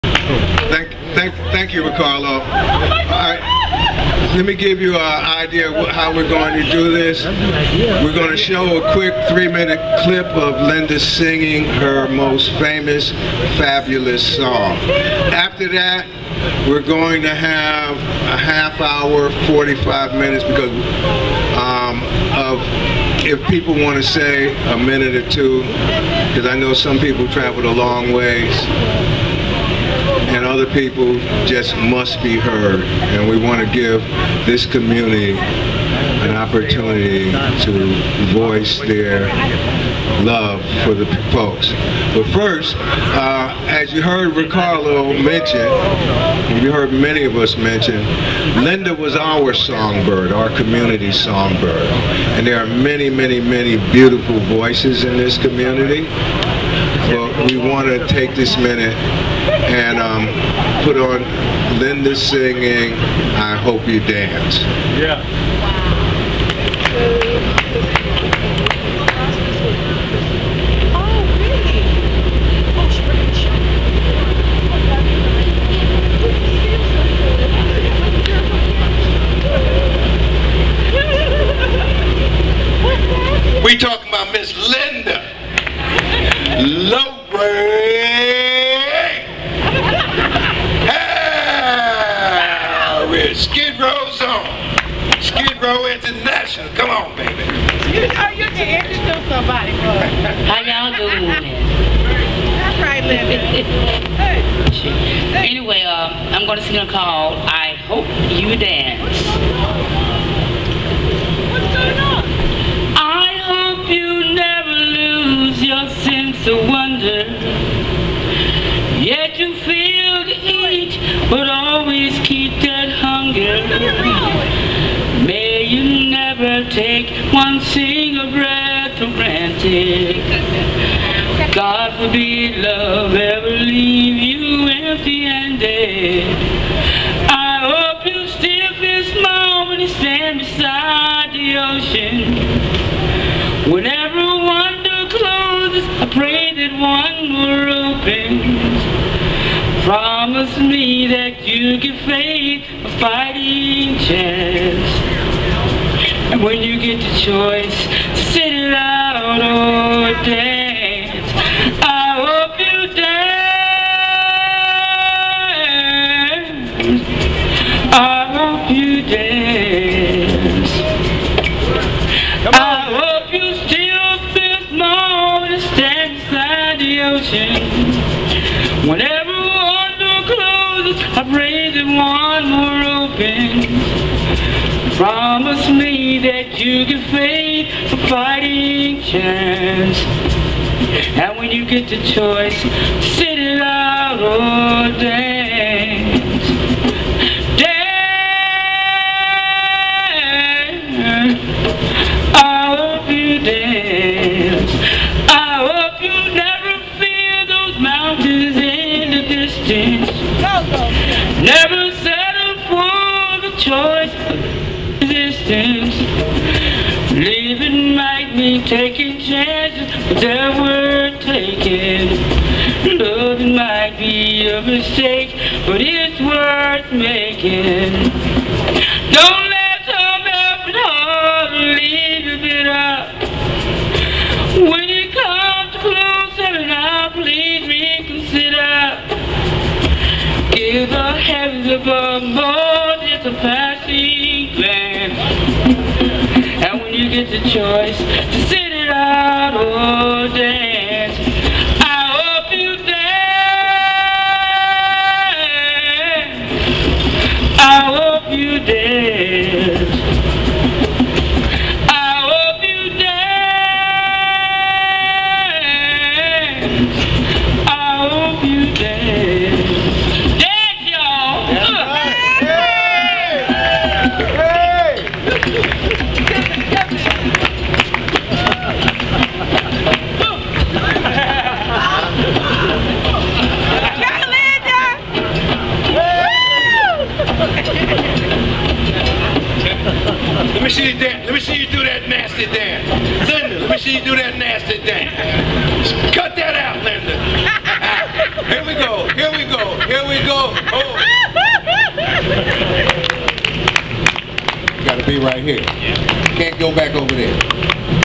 Vid. 6 Final Skid Row Movies In The Park Summer Series & Tribute to Linda Harris and Mollie Lowery
10.4.16 The feature movie was "Lost Angels: Skid Row Is My Home" a Documentary, a warm, humanistic portrait of Los Angeles' Skid Row. This was the final Skid Row Movies In The Park Summer Series & Tribute to Linda Harris and Mollie Lowery in Gladys Park in.